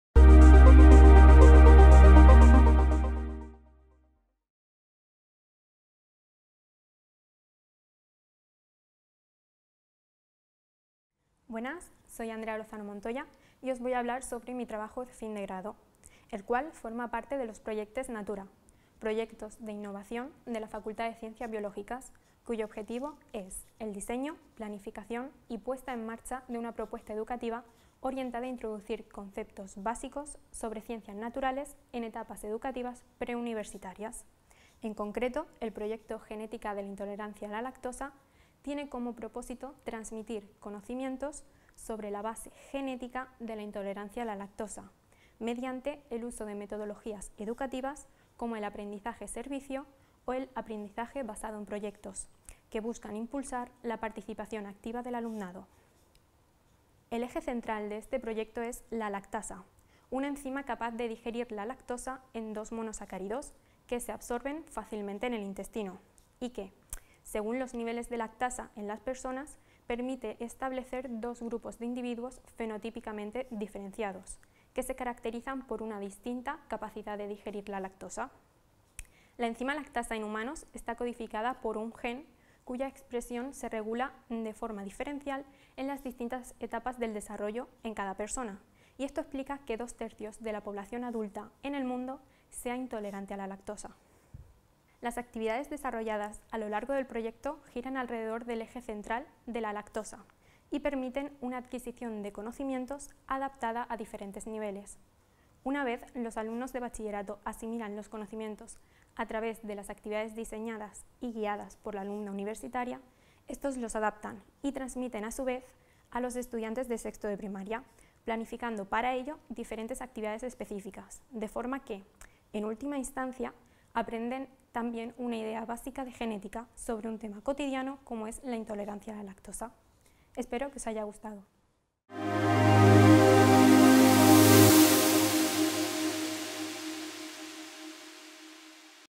GENÉTICA DE LA INTOLERANCIA A LA LACTOSA
Autora: Andrea Lozano Montoya

Este Trabajo Fin de Grado forma parte de los “Projectes Natura”, proyectos de innovación de la Facultat de Ciències Biològiques, cuyo objetivo es el diseño, planificación y puesta en marcha de una propuesta educativa orientada a introducir conceptos básicos sobre Ciencias Naturales en etapas educativas preuniversitarias. En concreto, el proyecto “Genética de la intolerancia a la lactosa” tiene como propósito transmitir conocimientos sobre la base genética de la intolerancia a la lactosa mediante el uso de metodologías educativas como el Aprendizaje-Servicio (ApS) o el Aprendizaje Basado en Proyectos (ABP) que buscan impulsar la participación activa del alumnado. El eje central de este proyecto es la lactasa, una enzima capaz de digerir la lactosa en dos monosacáridos que se absorben fácilmente en el intestino y que, según sus niveles en las personas, permite establecer dos grupos de individuos fenotípicamente diferenciados que se caracterizan por una distinta capacidad de digerir la lactosa. Las actividades desarrolladas y los conocimientos adquiridos a lo largo del proyecto giran alrededor de este eje y, tras su asimilación por los alumnos de 1º de Bachillerato, los adaptan y los transmiten a estudiantes de 6º de Primaria para que ellos, en última instancia, también aprendan una idea básica de genética sobre un tema cotidiano como es la intolerancia a la lactosa.

Licencia
Licencia Atribución de Creative Commons (reutilización permitida)
Licencia